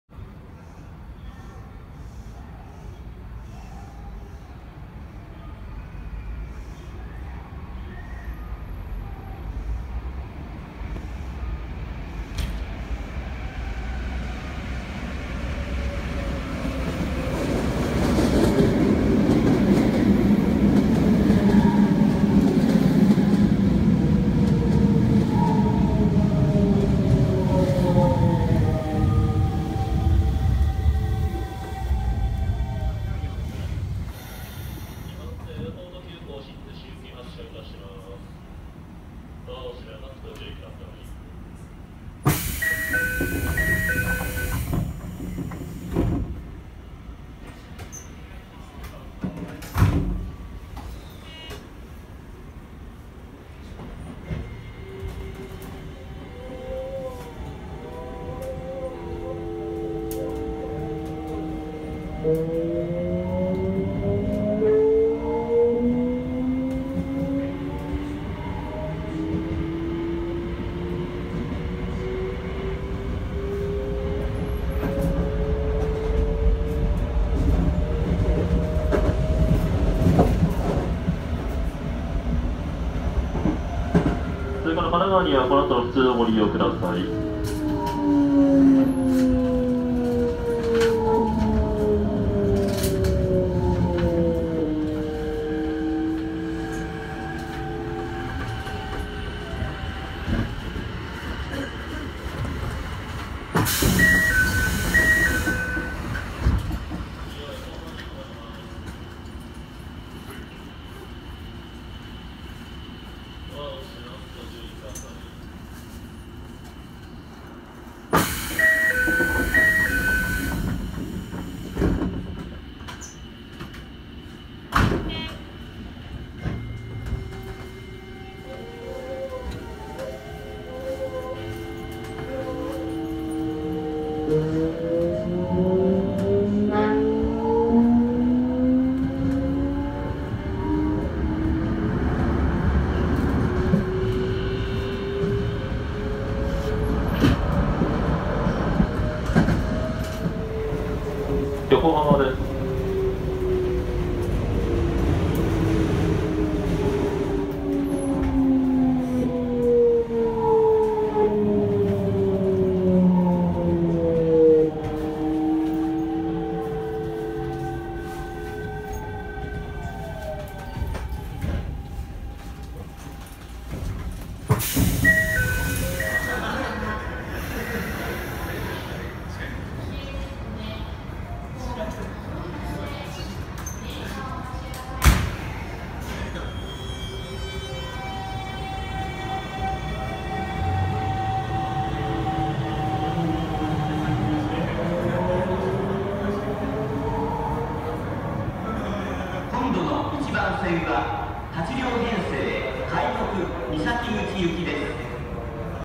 【三菱GTO】東京都交 5300形5312F走行音(新逗子エア急) / Toei-5300 sound
Japanese train running sound and view in the car.(TOKYO/Toei-Subway/Series5300/MITSUBISHI-VVVF)

都営浅草線用5300形5312Fの京急線内新逗子エアポート急行走行音です。VVVFは三菱GTO後期タイプで、まさに爆音サウンドといった磁励音です。

・京急線内から帰らない休日41T
現行ダイヤの休日41T運用は、朝に印西牧の原から羽田空港入りした後は終日羽田空港↔︎新逗子をエアポート急行で往復し、夜に金沢文庫に入庫します。京急鶴見はじめ横浜市内各駅では時折やってくる都営地下鉄の車両が新鮮に映ります。

・本線快特への制約
京急蒲田以南の5300形本線運用は、エアポート急行運用を除くと朝夜の特急運用のみです。(例外的に平日夜に三崎口発の上り快特が1本あり)
これは最高速度120km/hの日中の快特ダイヤに最高110km/hの5300形が乗れないことと、ラッシュ時に12両となる本線快特に5300形が投入出来ない点にあります。これらの制約が新型5500形で解消されるのか注目したいところです。